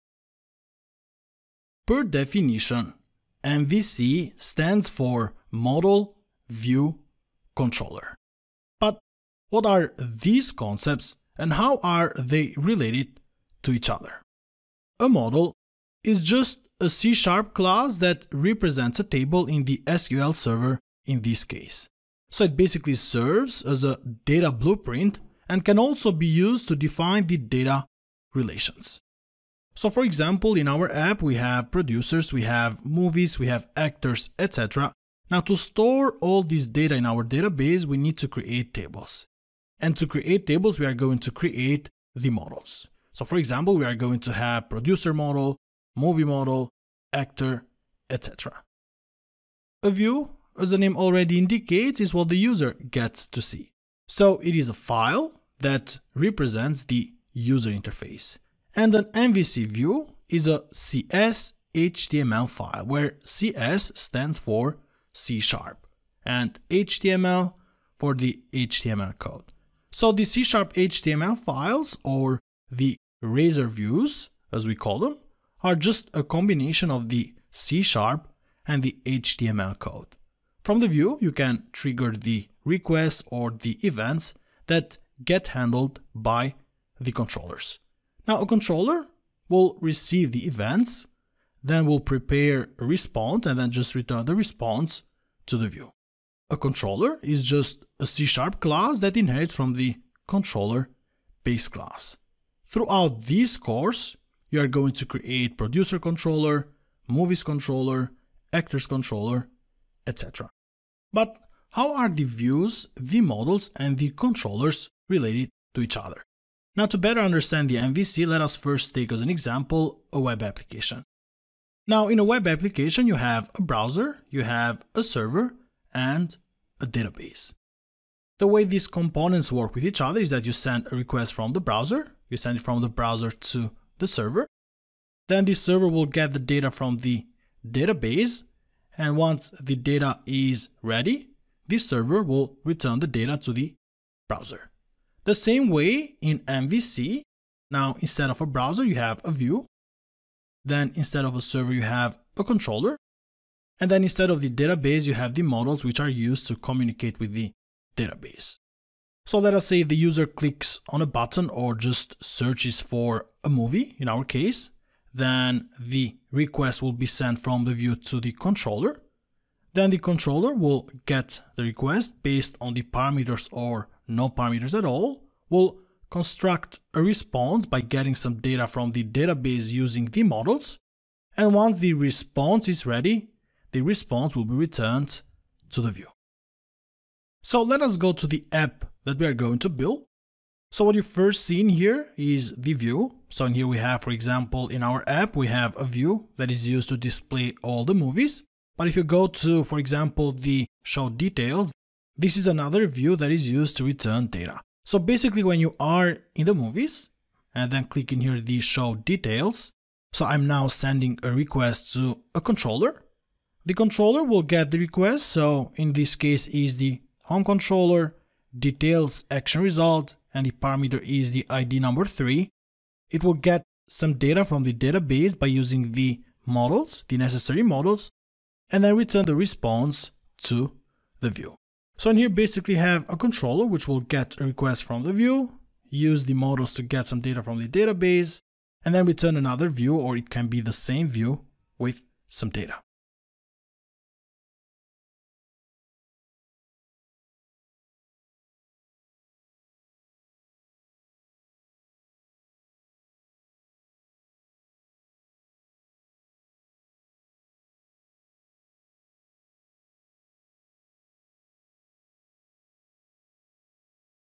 03. What is ASP.NET MVC?
In ASP.NET MVC, MVC stands for Model-View-Controller. A model is used as a data signature to interact with the database, a view is what the users sees and a controller is a C# class responsible for the application logic. ASP.NET MVC is a great framework to easily separate the application concerns.